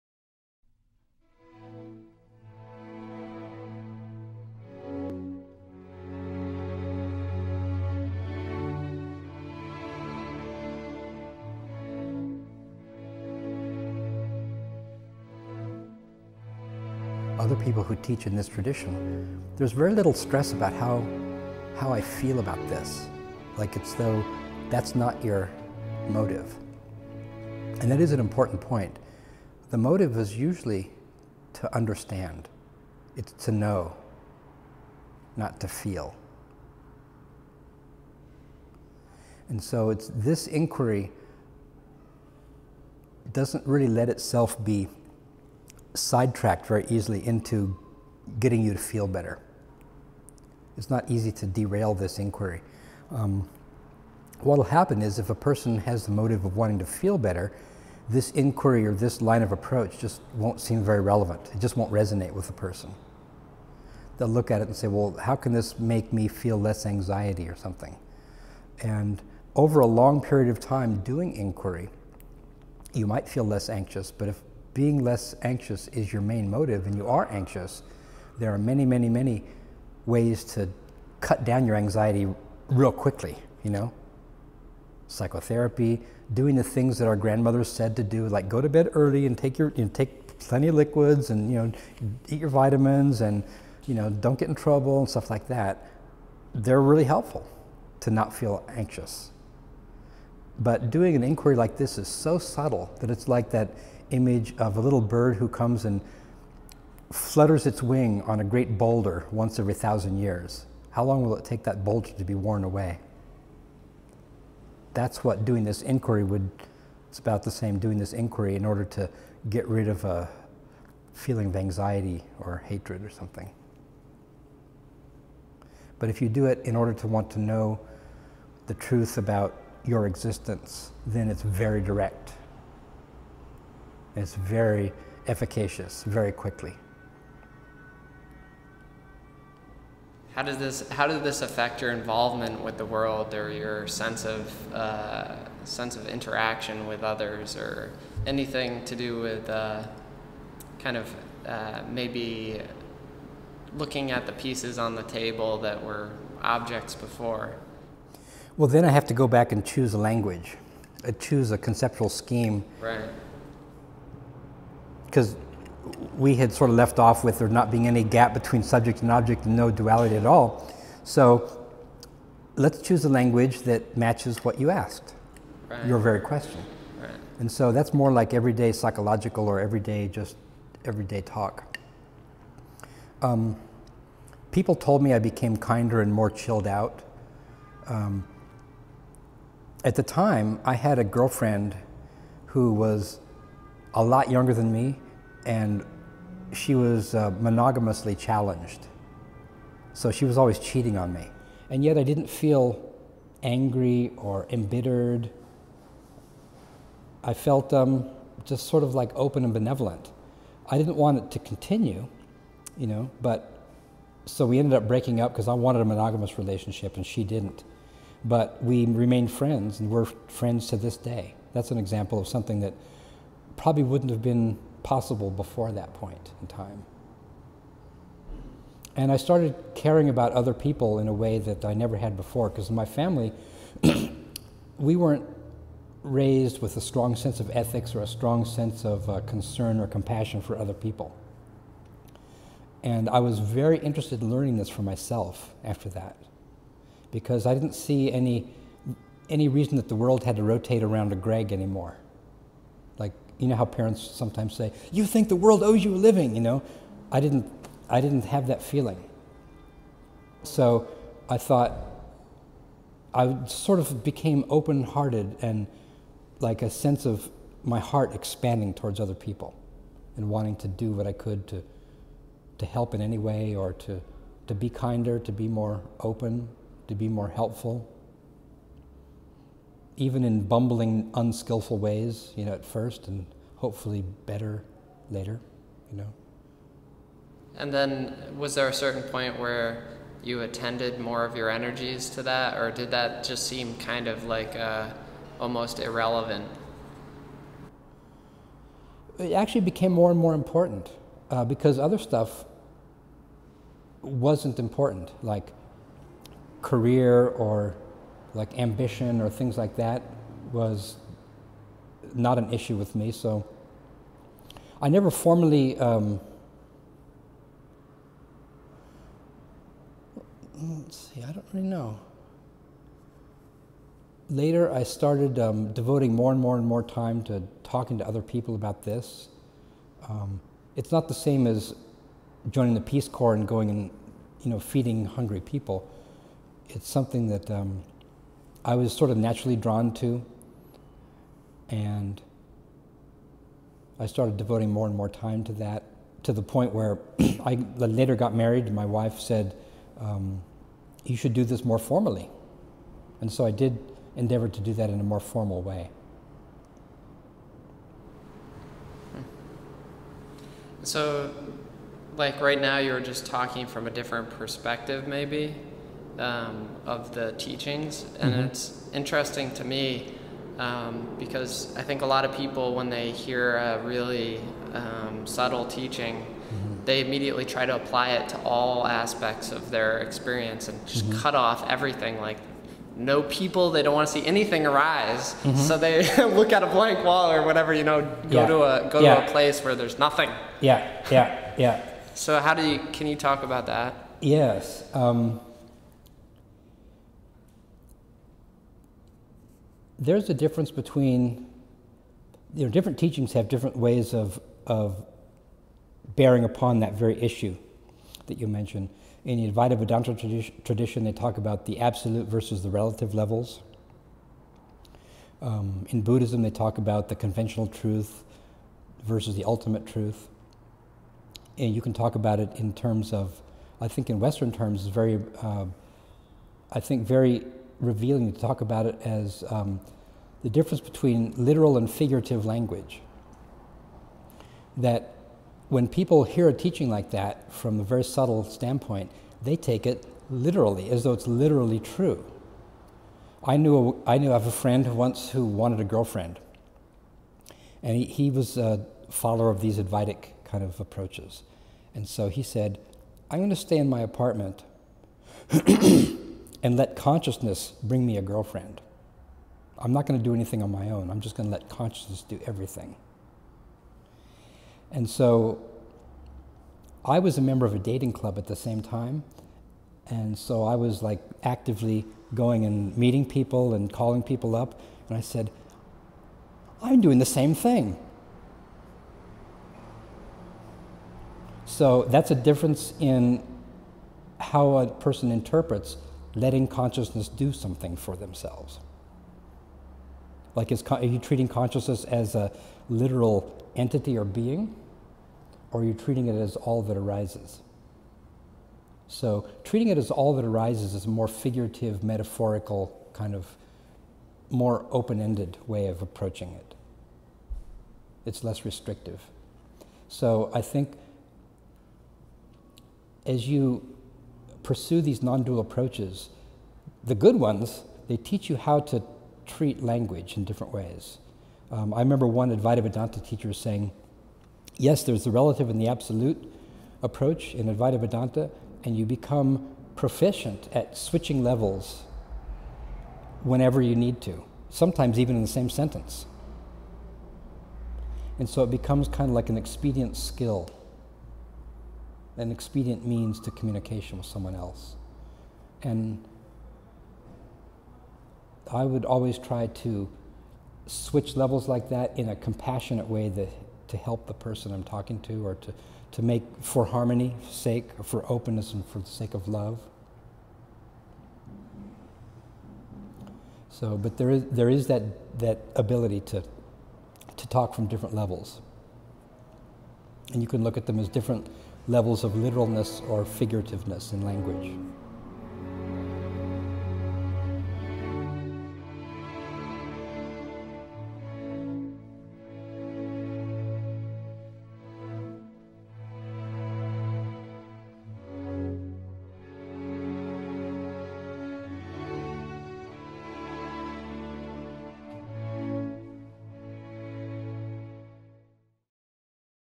Literal and Figurative Language - Greg Goode
Discusses the various motives one might have for pursuing nondual teachings.  Also discusses how nondual teachings have literal and figurative ways of using language. Truth with non-dual teacher, Greg Goode. - Filmed by Roger Ingraham